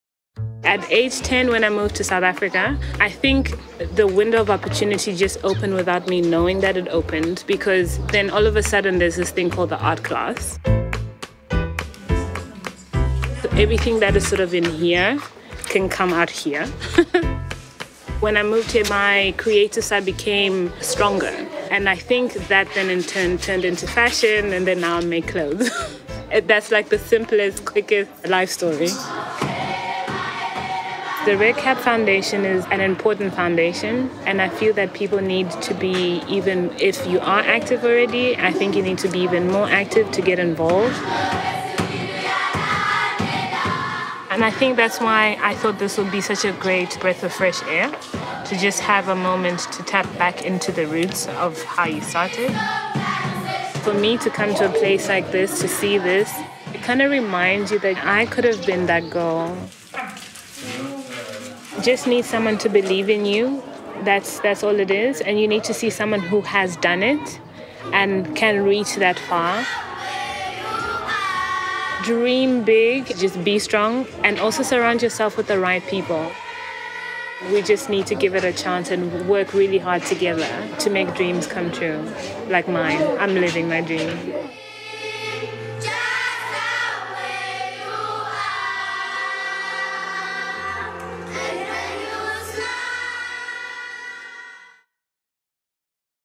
Spotlight on South African designer Anisa Mpungwe
Anisa Mpungwe, winner of the 2008 Elle New Talent Design Award and creator of Loin Cloth and Ashes has teamed up with Mr Price to create a visually magnetic, limited-edition range.

Anisa's journey with Mr Price began in 2008 when she won the Elle New Talent Design Award in association with Mr Price. Since winning this prestigious award, Anisa's career has gone from strength to strength.

Joanne Frédéric, Trend Director of Mr Price says, "We've always admired Anisa's creativity, style, tenacity, and her passion for nurturing young talent and giving back to the community."

The RedCap Foundation is a cause close to Anisa's heart. Established in 2006 as a registered non-profit and public benefit organisation, the Foundation aims to make a positive difference in the lives of young South Africans. Schools in South Africa, particularly those in less privileged communities, face a range of challenging obstacles. The Foundation's Red Cap Schools project is a direct response to these challenges.

 Anisa recently visited one of the schools that the Foundation is working with to empower young children. Upon visiting the school, the talented designer was visibly moved,

"To come to a place like this reminds me of my roots. You just need someone to believe in you. We need to work really hard together to make dreams come true. I'm fortunate to be living my dream."

Anisa showered the little ones with hugs as they danced around her in their classroom! Together, they drew pictures with crayons, just as Anisa did when she was a little girl. "Art was an important part of my childhood. I want our children to be creative and follow their hearts."

Spending time with the children took Anisa back to her childhood, and reminded her of how far she has come. As they huddled around her, Anisa encouraged the children to "Dream big, be strong and surround yourself with the right people."